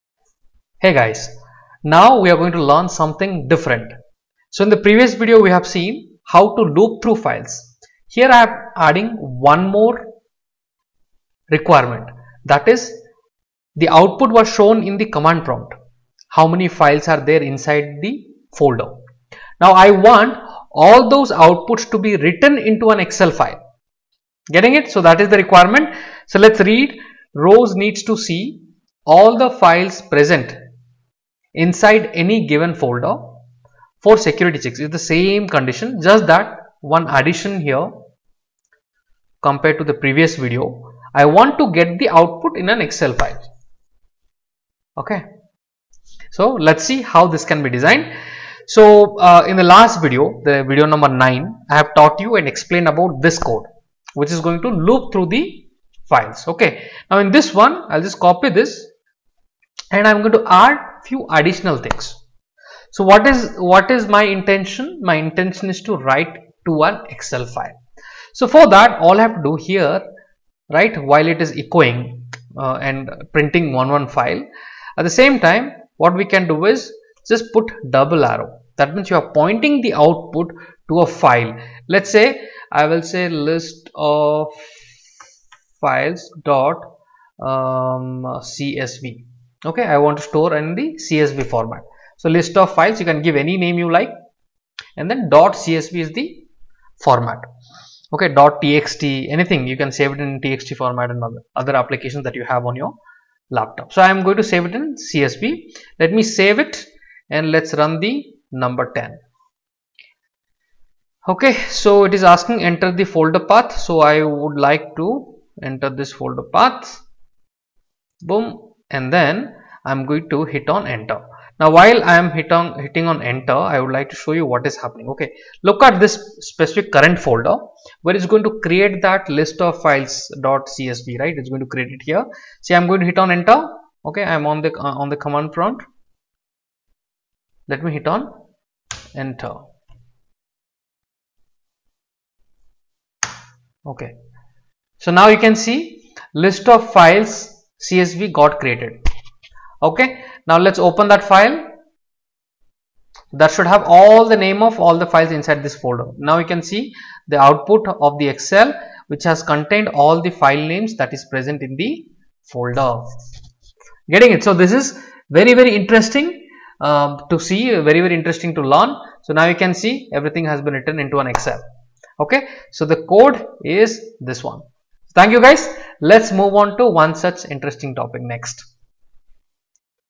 Batch File Loop Through Files and Write to Excel
Learn to design a Batch File Loop Through Files and Write to Excel. Check this interesting example.

A batch file is a script file that stores commands to be executed in a serial order. It helps automate routine tasks! A scripting language is a programming language that is created to interpret and execute one command at a time. Wish to be a Scripter you have visted the right place.